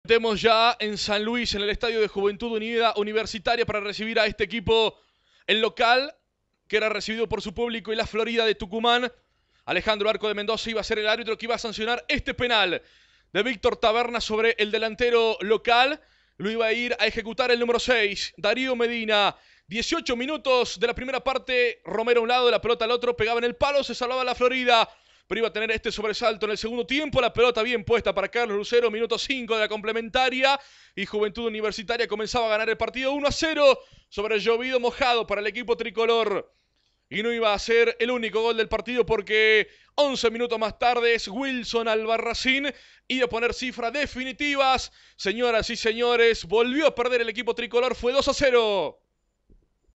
altoque | JUVENTUD SL 2-0 LA FLORIDA - ARGENTINO A
otra derrota del tricolor esta vez en san luis